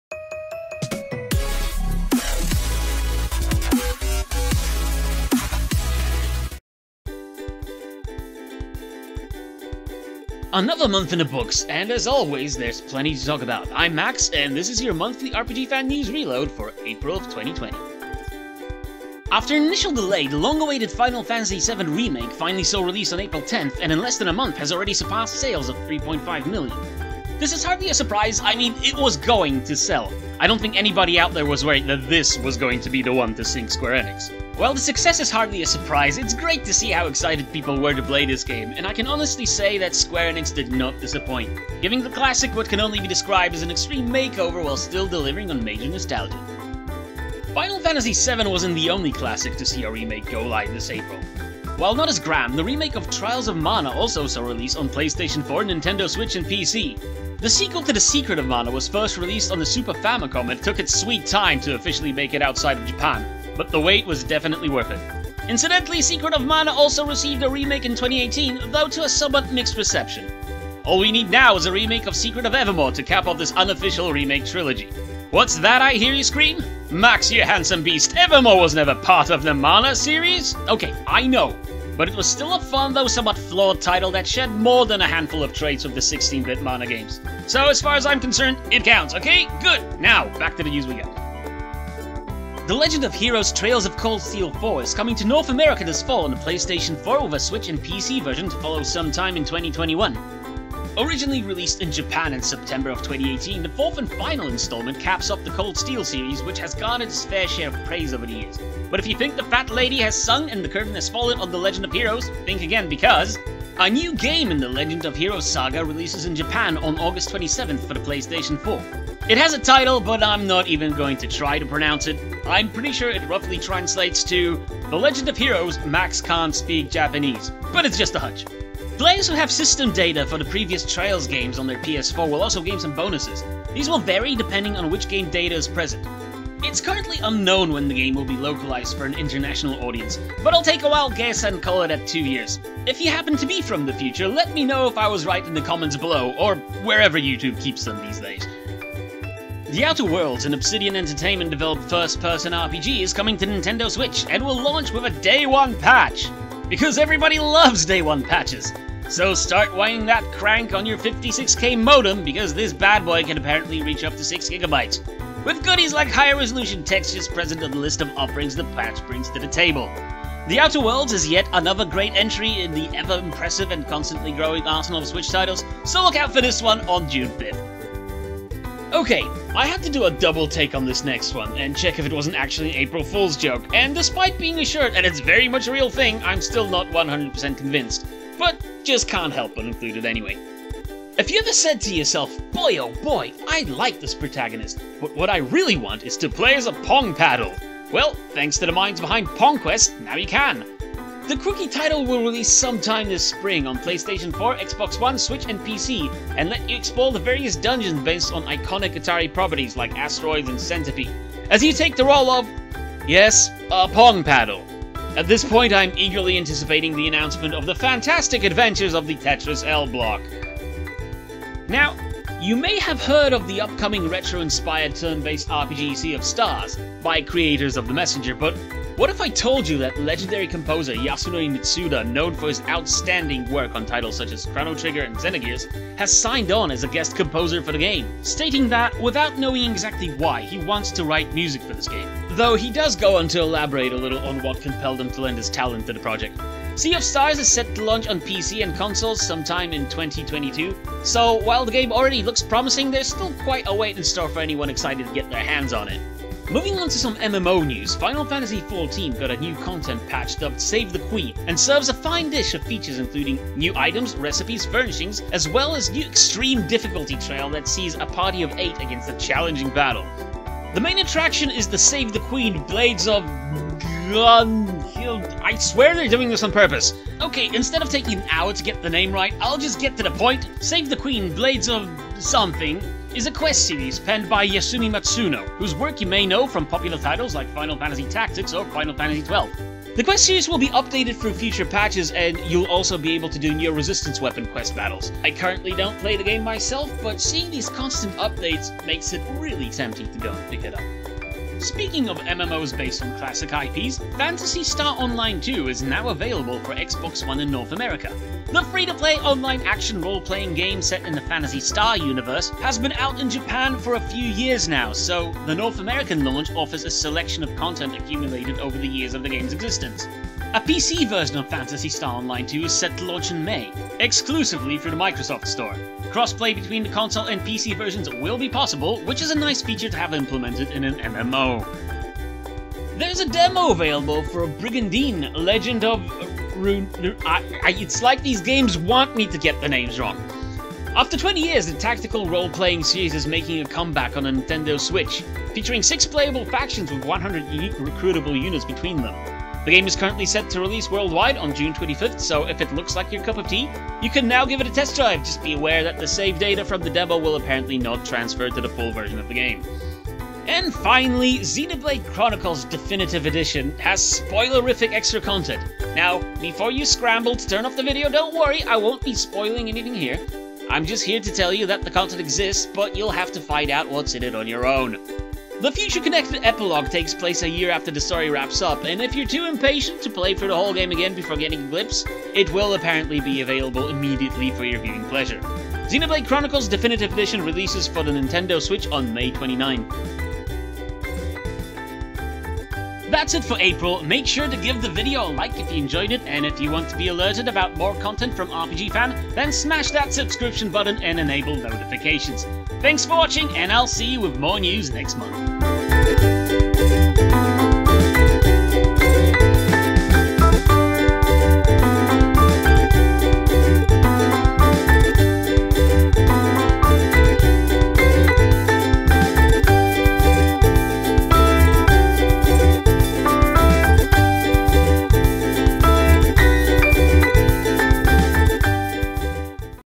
Trials of Mana, Xenoblade Chronicles Remaster and more - RPGFan News Reload - April 2020
April had a lot going on, so tune in and join Maxx as he runs down some of the top items!

Host: Maxx

RPGFan News Links:
Final Fantasy VII Remake Sales Top 3.5 Million

The Legend of Heroes: Trails of Cold Steel IV Coming West

The Legend of Heroes: Hajimari no Kiseki Releases in Japan on August 27th

The Outer Worlds Hits Nintendo Switch on June 5th

Sea of Stars Adds Yasunori Mitsuda as a Guest Composer

Final Fantasy XIV "Save the Queen" 5.25 Patch Available Now

Phantasy Star Online 2 Available Now for Xbox One

Brigandine: The Legend of Runersia Demo Now Available For Nintendo Switch

Xenoblade Chronicles Future Connected Epilogue Set A Year After the Game's Ending